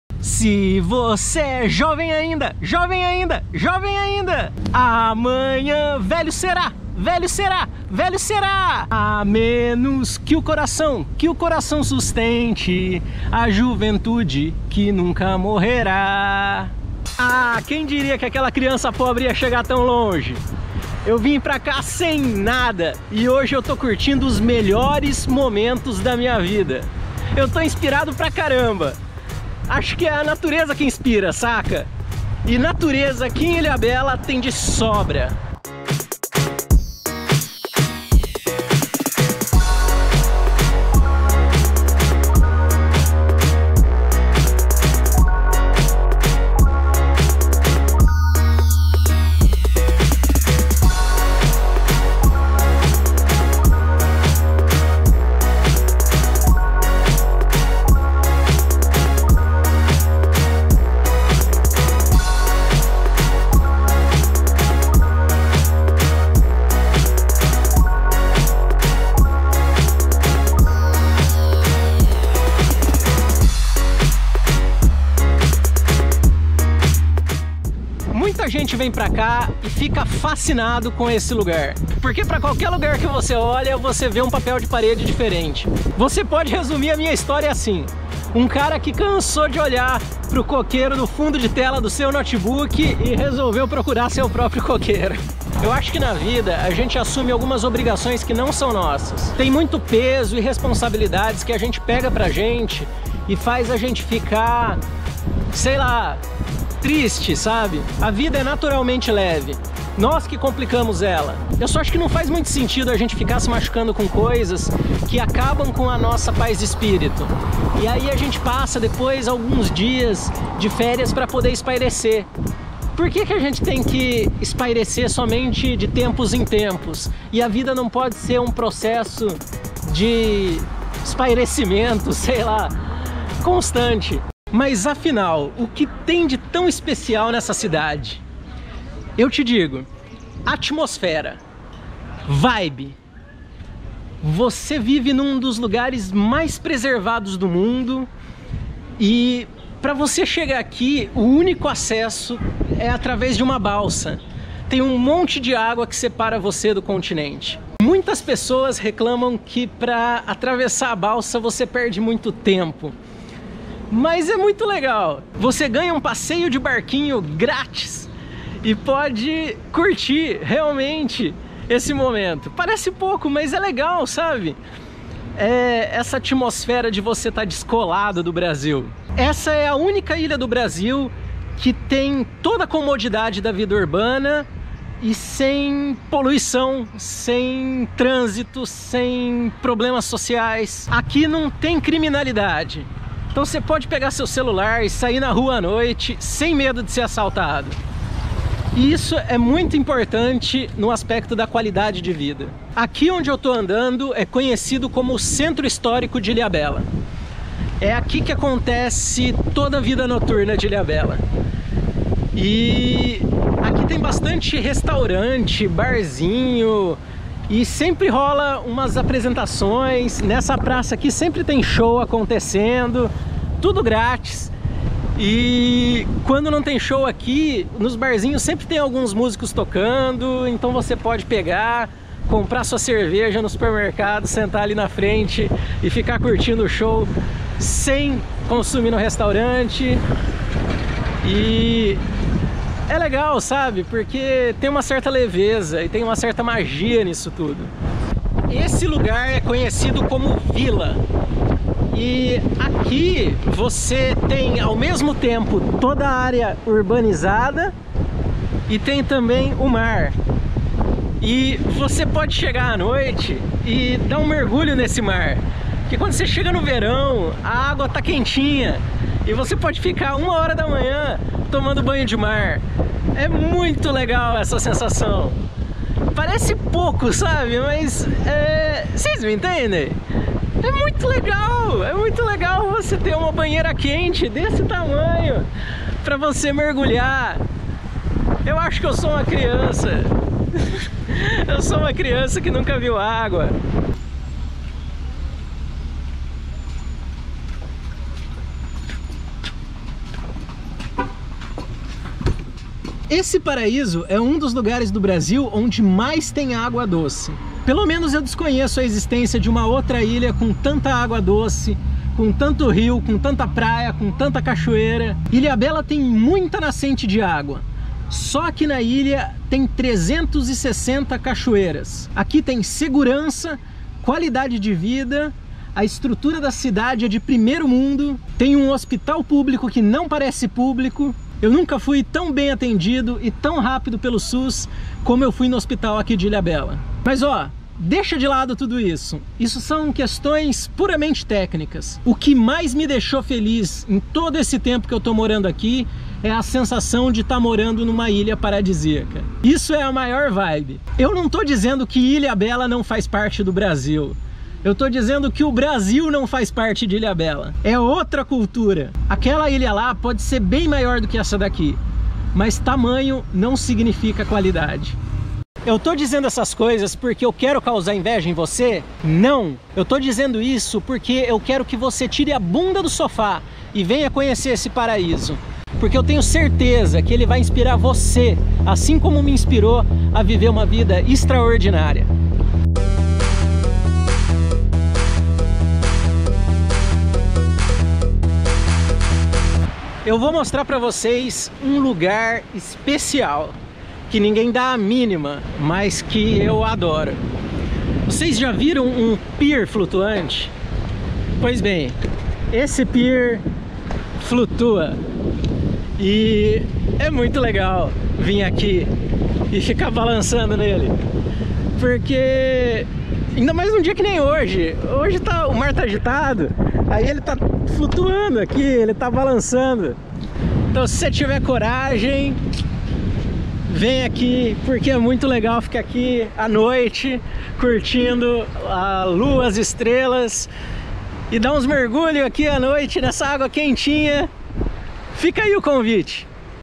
O paraíso do Brasil é aqui! | ILHABELA - São Paulo
Vim morar em um paraíso no Brasil, bem pertinho da capital de São Paulo, a cidade de Ilhabela é a maior ilha paradisíaca do Brasil. Minha experiência em morar nesse lugar está sendo incrível, principalmente pelo contato diário e constante com a natureza numa cidade com mais de 360 cachoeiras além de dezenas de praias paradisíacas.

Ilhabela possui a maior renda per capita do Brasil e é um dos únicos municípios arquipélagos do Brasil. Localizado no litoral norte de São Paulo, na microregião de São Sebastião, seu acesso é através de uma balsa em uma travessia de 15 minutos para carros e pedestres.

Vale a pena conhecer esse pedaço de paraíso na terra que possui 2 das praias mais lindas do mundo: a Baía dos Castelhanos e a Praia do Bonete.

Tem vídeo toda semana. Ative as notificações! ✌️